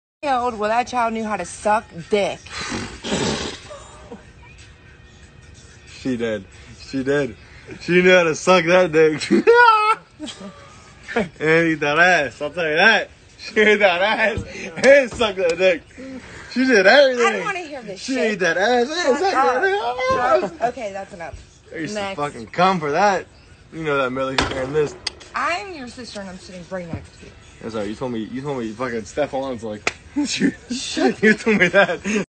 Hunter Echo admits to grooming Millie Bobby Brown
that's sick to talk about a child like that